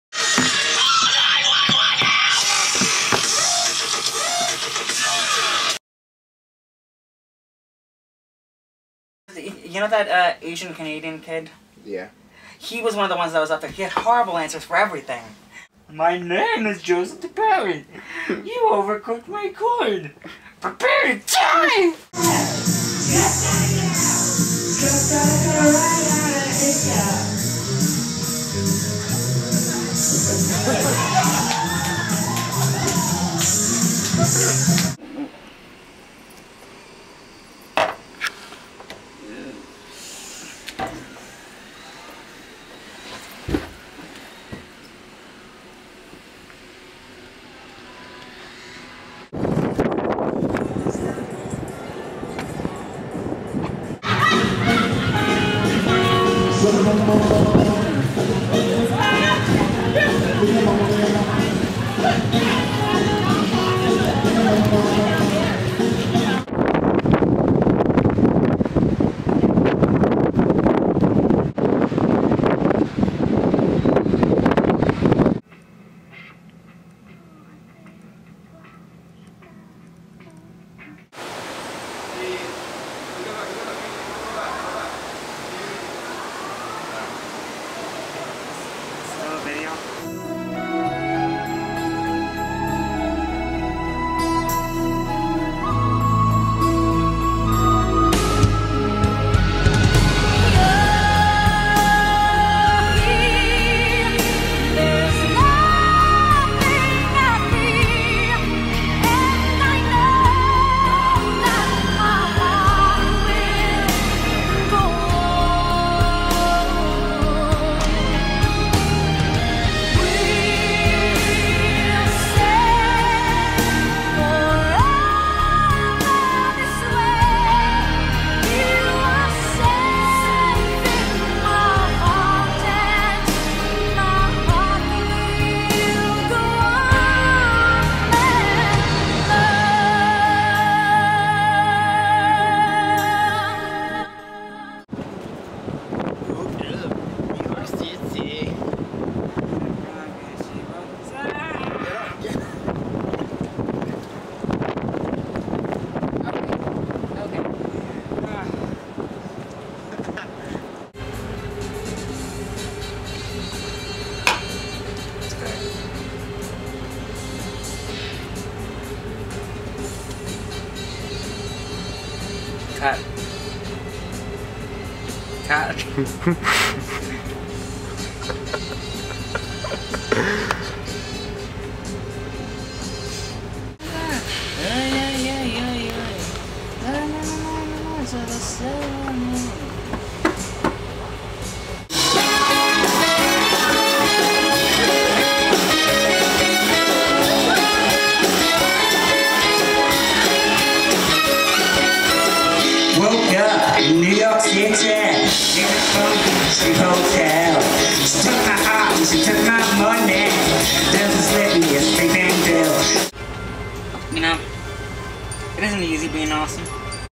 It Ain't Easy Being Awesome
Compilation of Joseph DiPeri during our cruise from August 25- September 2, 2012